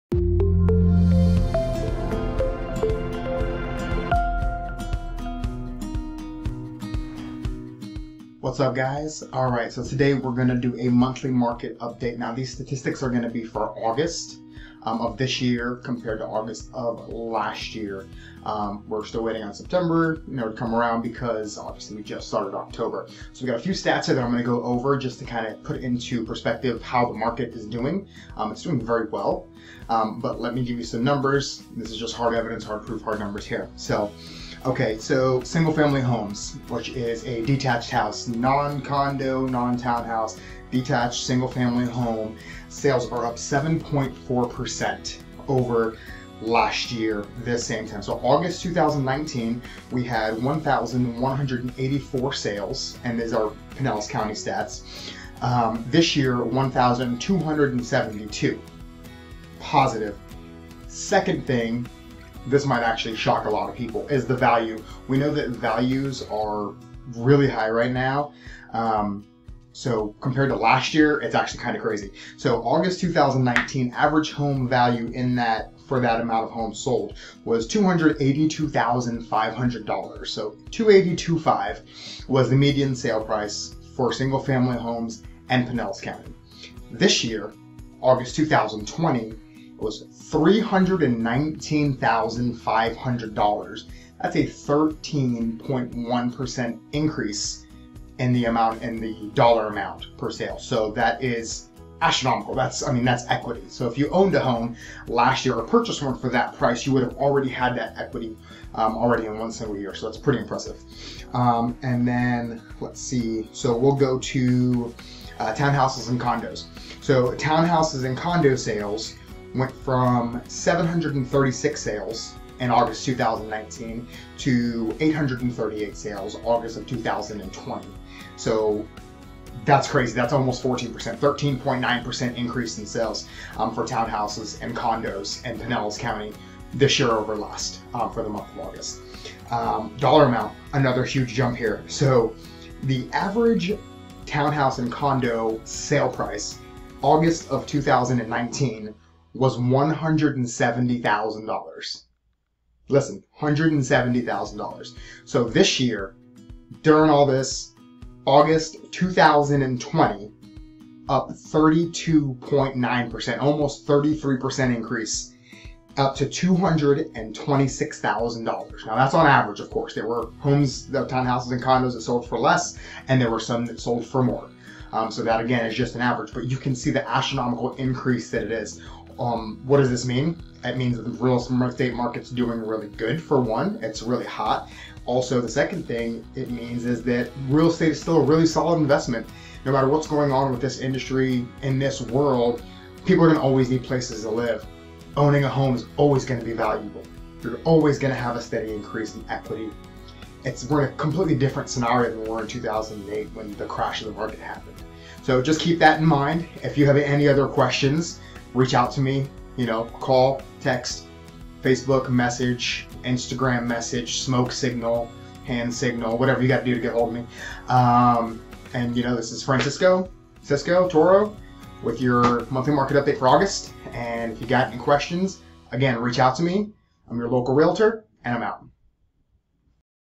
Florida Real Estate Market Update - August Pinellas Market update (THE NUMBERS SAID WHAT!)
In this video I compare August 2020 to August 2019. I will cover sales and home values for Single family homes & Townhomes/Condos. I think you'll be shocked.
WATCH NOW!
(If you somewhere loud, don't worry it closed captioned)